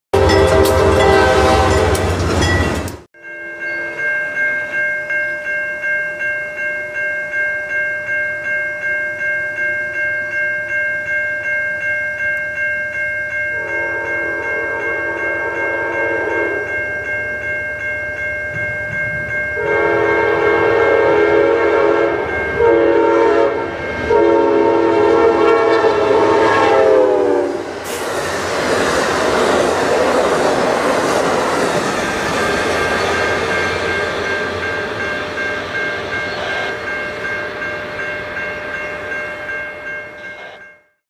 Amtrak Capitol Limited at Swanton Ohio (Drone Video)
On a cool winters evening, Amtrak train 30, the eastbound capitol limited, races by at Swanton Ohio with a shortened winter consist. This consist reflects the lower ridership of the season.

Train: Amtrak #30, The Capitol Limtied
Locomotives: AMTK #(NA)
Cars: 6
Location: Swanton, Ohio
Mile Post: CP 307, Norfolk Southern Chicago Line
Date: 2/16/18